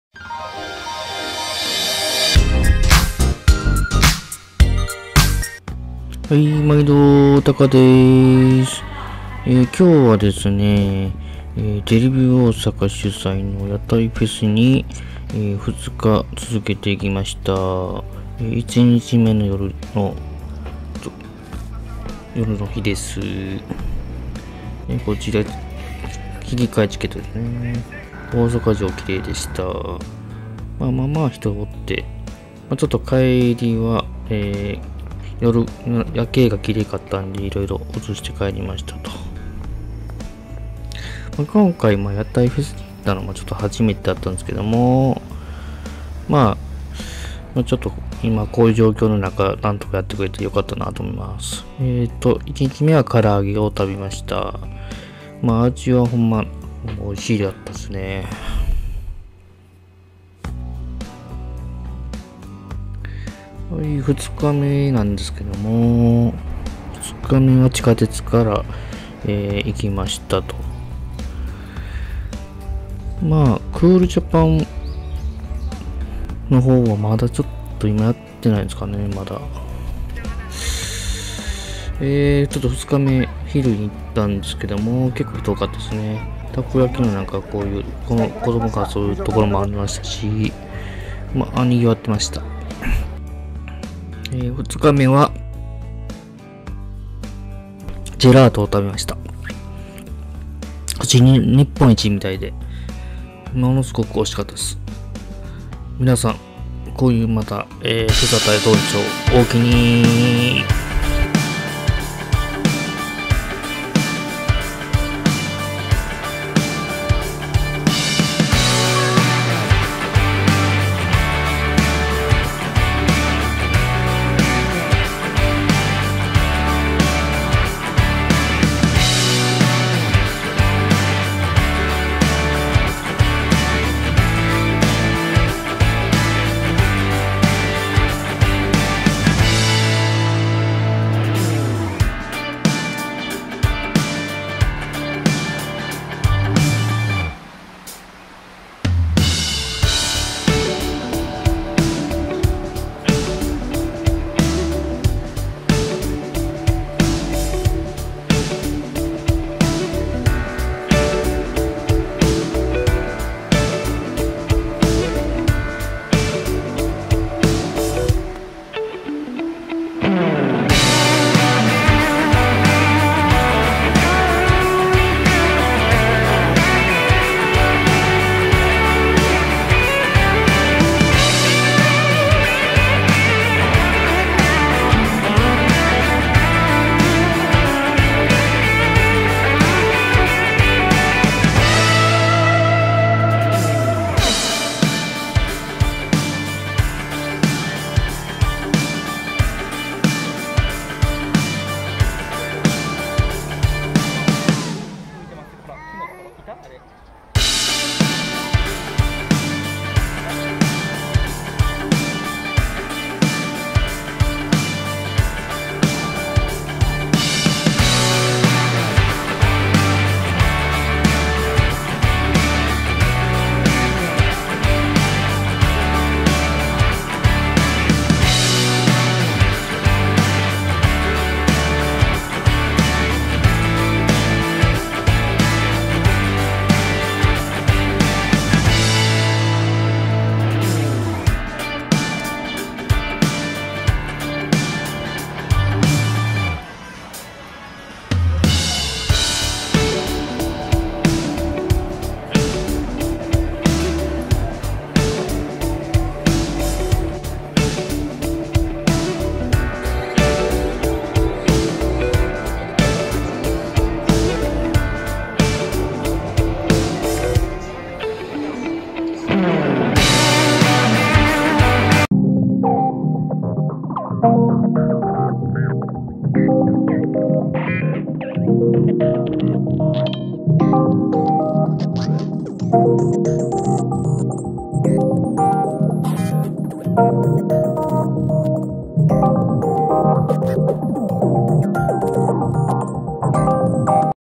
YATAIフェスに行ったよぃ。
撮影日時：2020年9月18日～19日
撮影場所：大阪城公園

0:00　オープニング
0:05　YATAIフェス
1:50    おまけ

YATAIフェスに行ってきました。
2日続け行ってきたよ。

このご時世の、
百貨店で物産展とかほとんどやってないので
ほんま有り難い。

▼撮影機材の紹介です▼